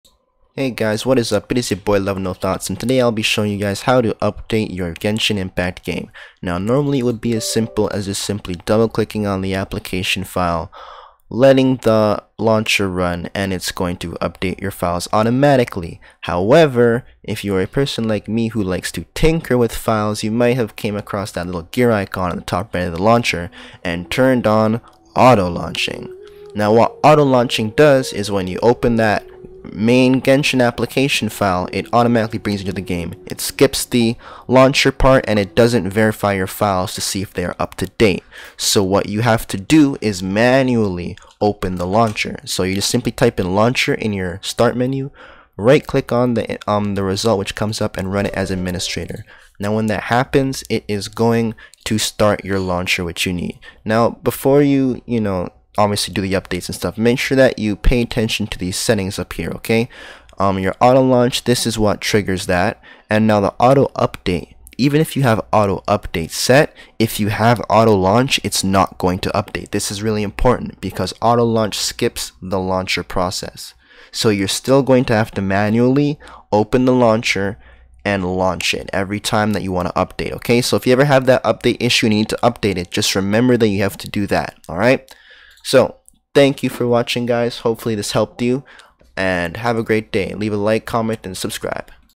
HOW TO FORCE GENSHIN LAUNCHER TO OPEN & UPDATE GAME! (PC)
So today I launched genshin and i couldn't update due to auto-launch being enabled. At first i was looking around in the directories to see if there was a config.ini setting i could change but it turns there is a main launcher file which you can execute!

I Also stream Genshin & Pokemon daily on twitch so leave a follow